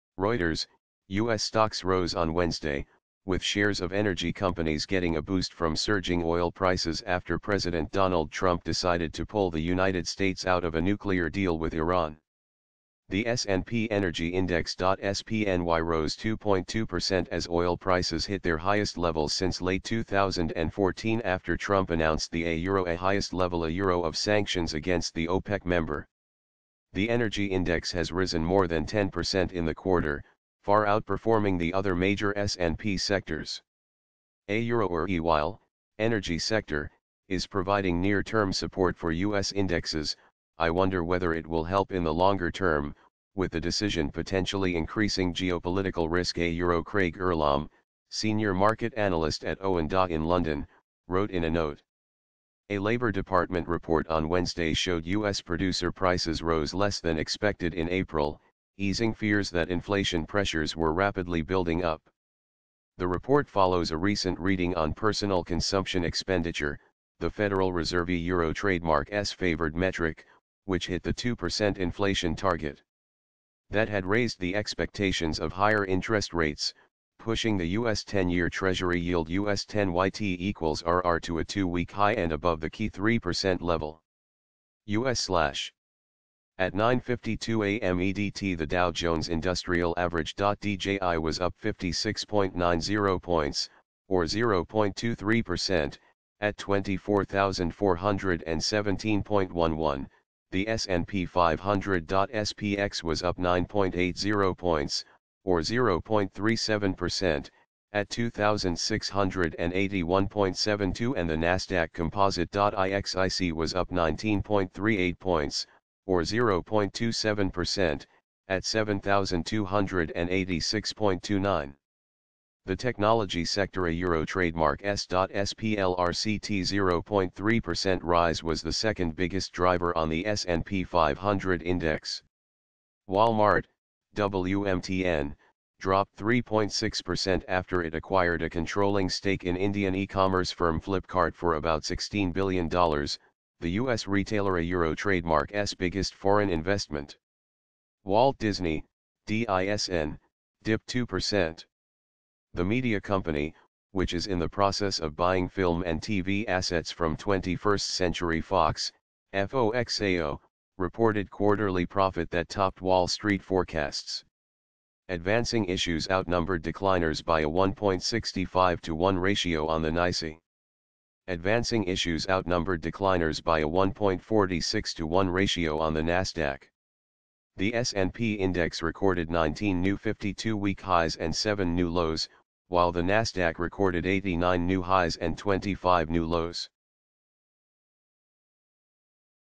Wall Street Rises As Oil Rally Boosts Energy Stocks | News Today | 05/09/2018 | Donald Trump
U.S. stocks rose on Wednesday, with shares of energy companies getting a boost from surging oil prices after President Donald Trump decided to pull the United States out of a nuclear deal with Iran.